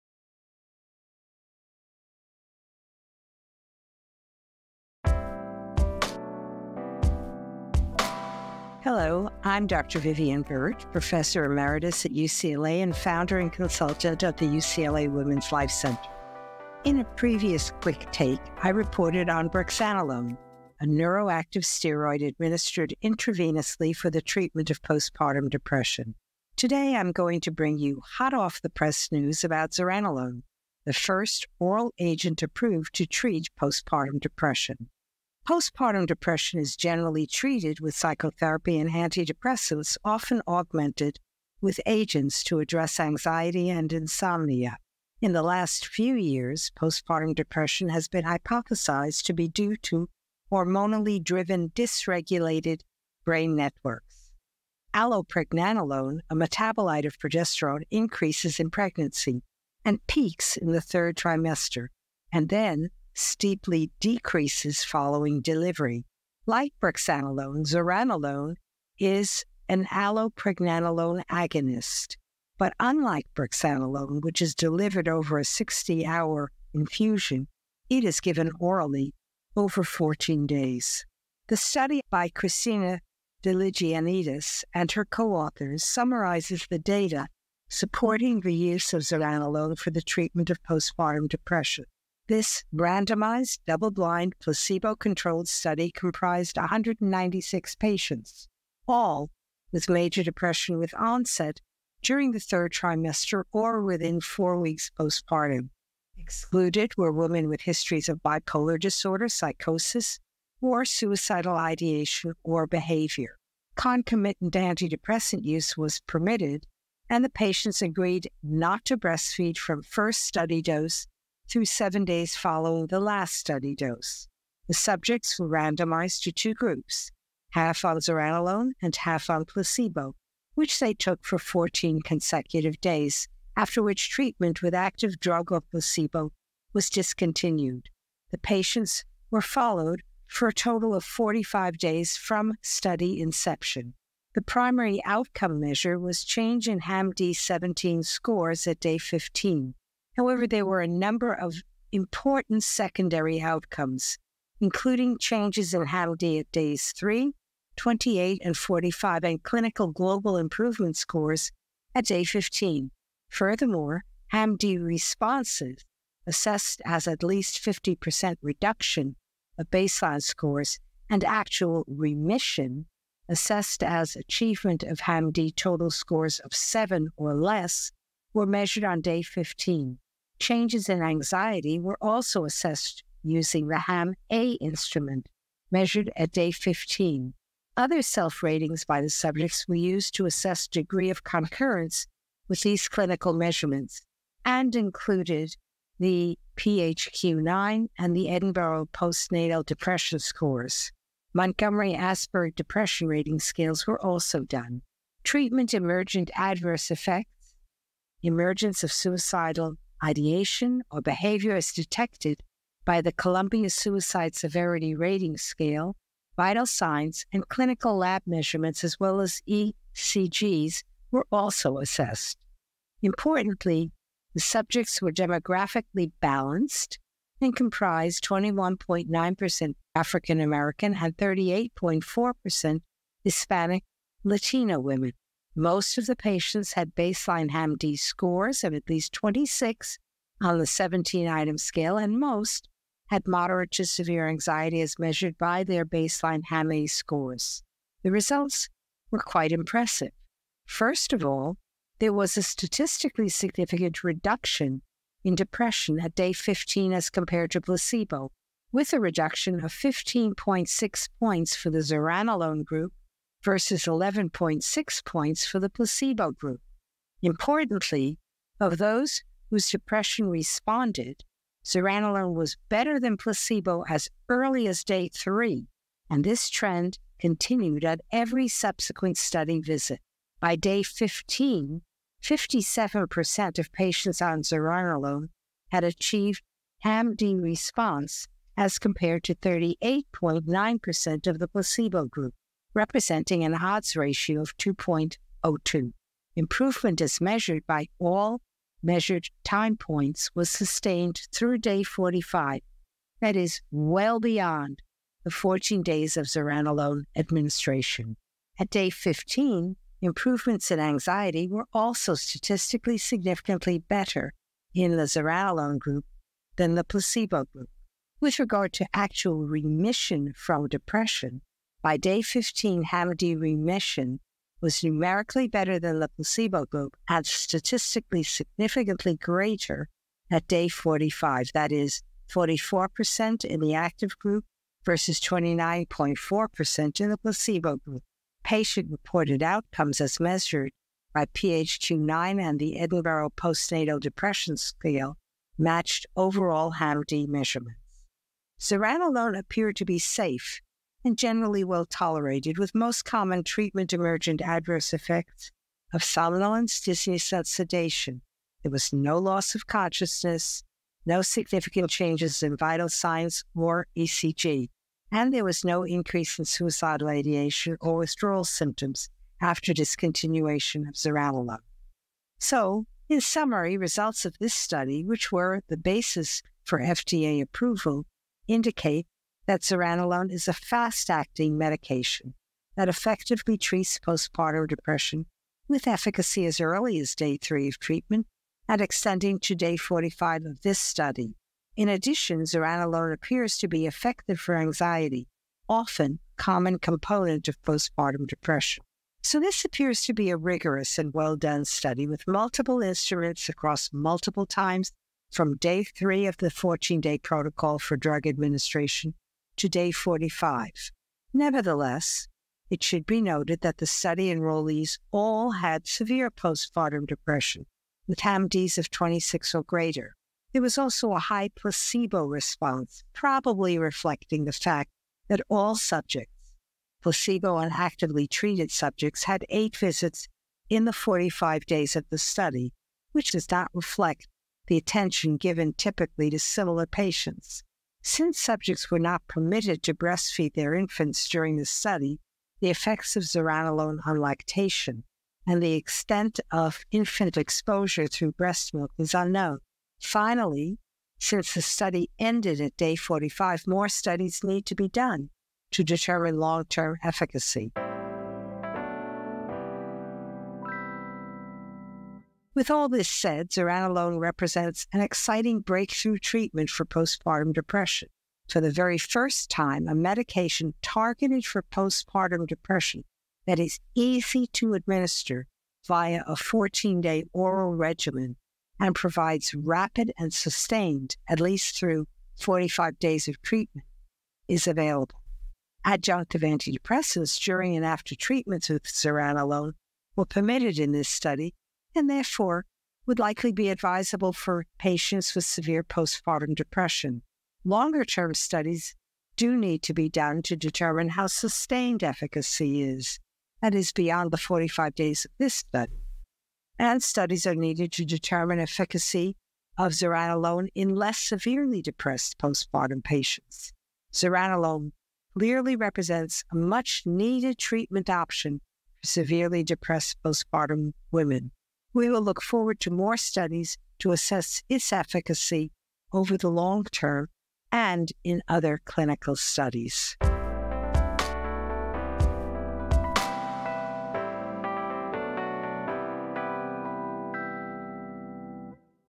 Zuranolone for the Treatment of Postpartum Depression
In this Quick Take, Dr. Vivien Burt discusses the safety and efficacy of zuranolone, the first oral allopregnanolone agonist indicated for the treatment of postpartum depression.

Did we mention you can also earn CME credits? This is something that you can take advantage of as a Silver or Gold member. Want to know more about Quick Takes and memberships?